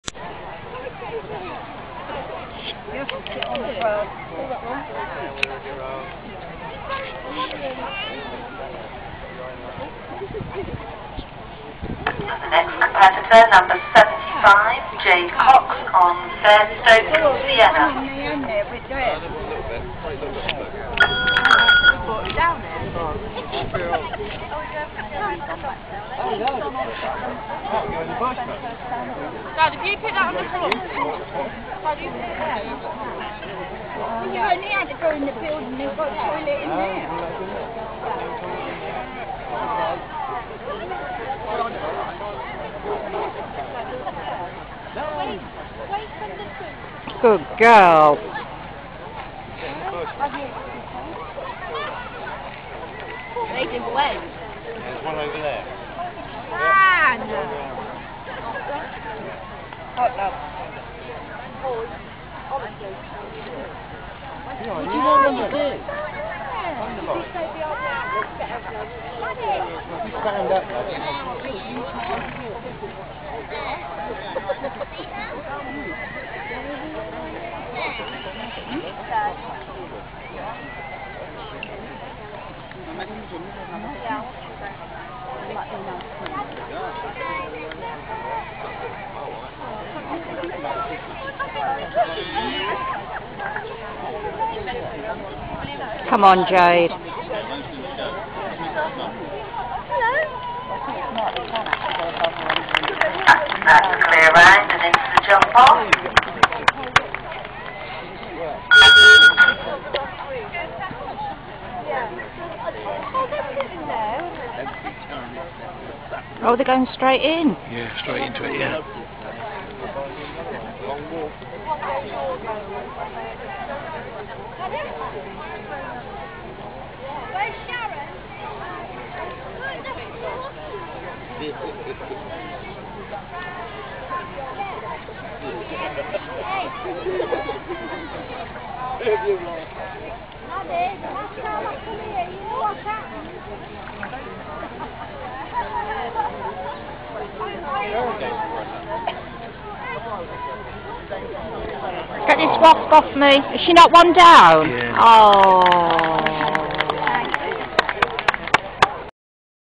Barbie 2nd in the 2'6ft
They said the would be a seperate jump off, i only found out it was two phase as i went into the ring so i had to learn it straight away. Couldnt remember where the first jump was! August bankholiday 2011. Had one pole down - the last one in the jump off. Were fastest 4 faults and there was only one double clear so we came 2nd. Smallest pony in the class by atleast 2 hands!!